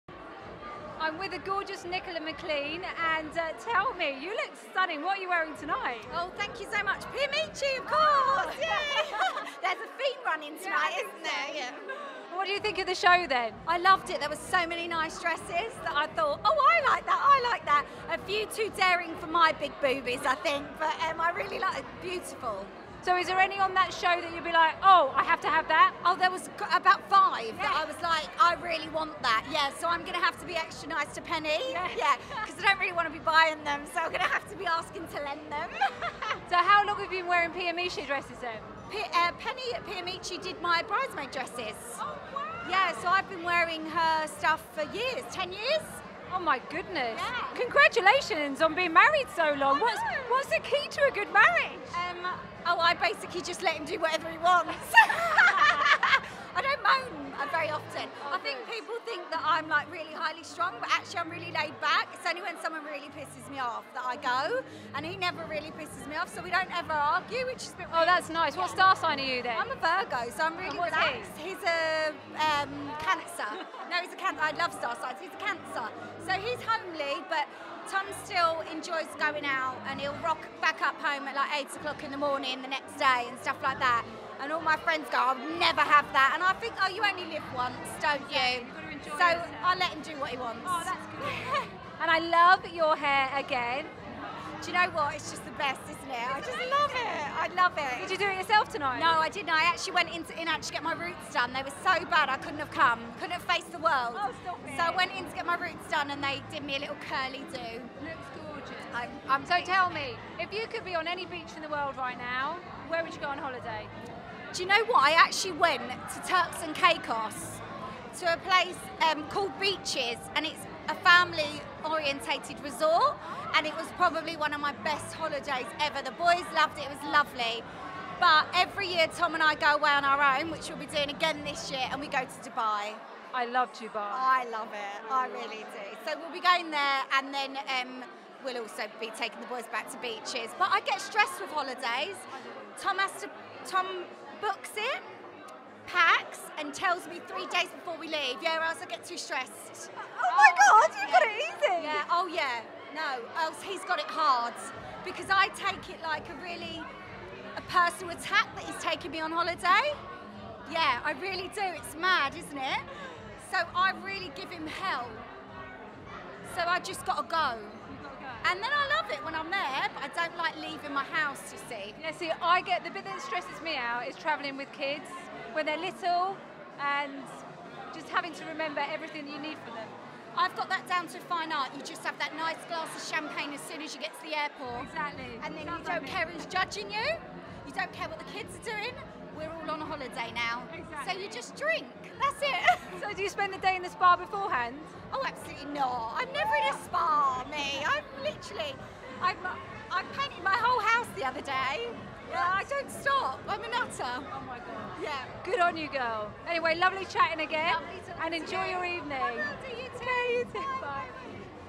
Naomi Isted interviews Nicola Mclean
Showbiz, Style and Beauty presenter Naomi Isted interviews model and presenter Nicola Mclean.

Both the ladies are wearing Pia Michi and have Gold Class Hair by Inanch London.

At the Pia Michi and Inanch London Catwalk Show in aid of Barnardos Mon 31st March 2014!! Millennium Hotel Mayfair. Filmed by Blondie Media Productions featuring " The Ultimate Lifestylist"

Follow Naomi for more hot Showbiz News, Celebrity Gossip, Style Trends and Beauty Reviews:

Follow Naomi for more top tips on Fashion and Beauty on Instagram and Twitter @naomikisted